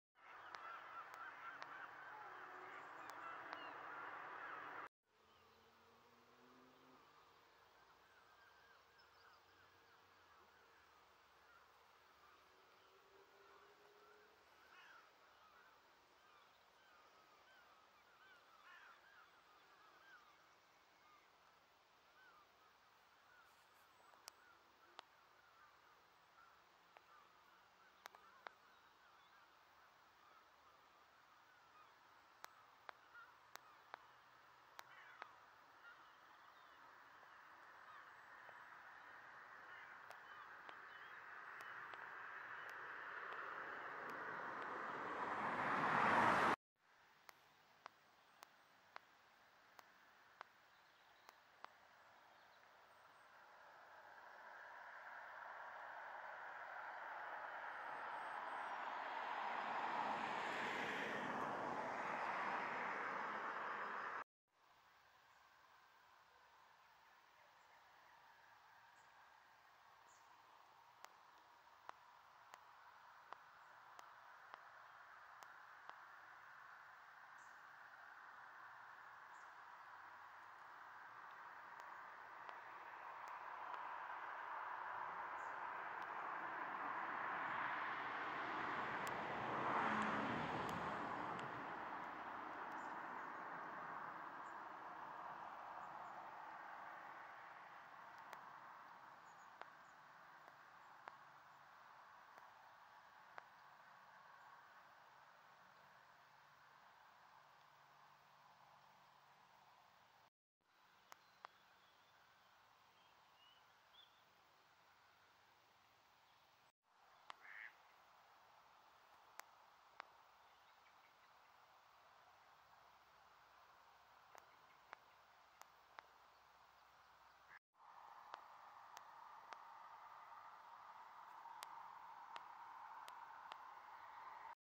Trowbridge Wetlands Project, Westwood Road, Trowbridge. Warning ⚠️ Flashing images.
Actually, there is no project. This is the result of geo-engineering. 3.3.24